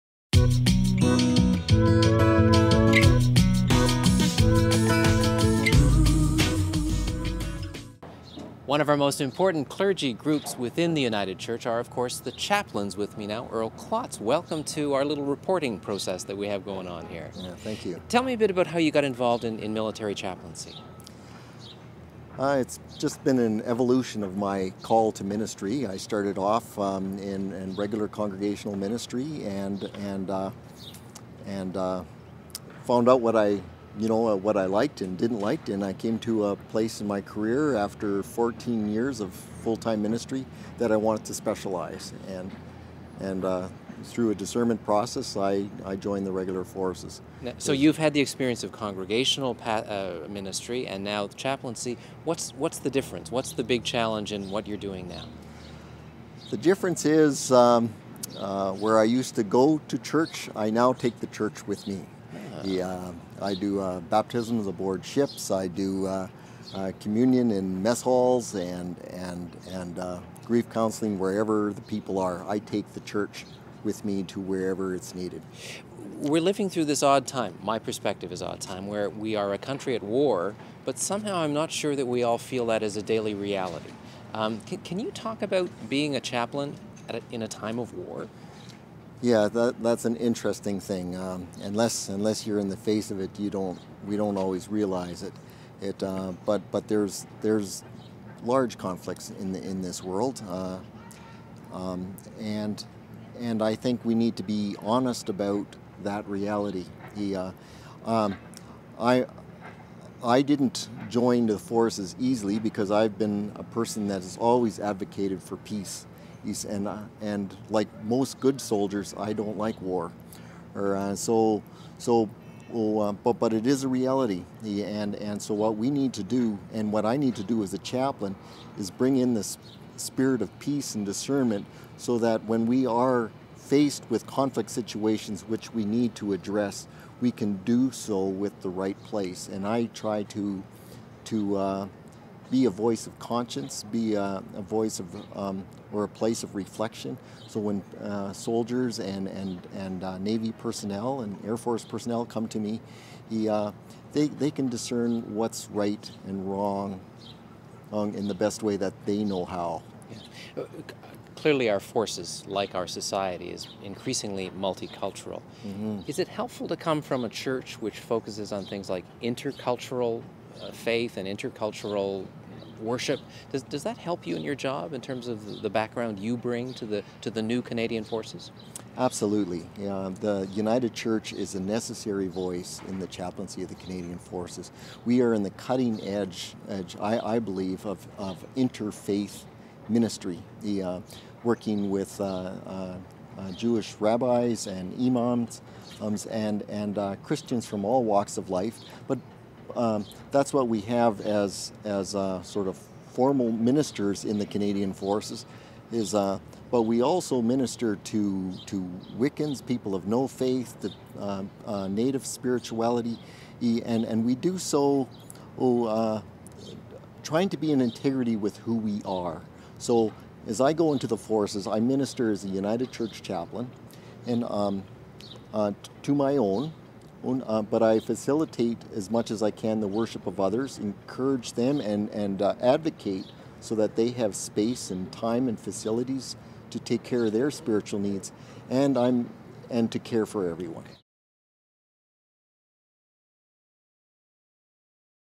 The church in a time of war
A conversation with Rev. Lt. (N) Earl Klotz, a United Church minister and chaplain in the Canadian Forces, based in Halifax.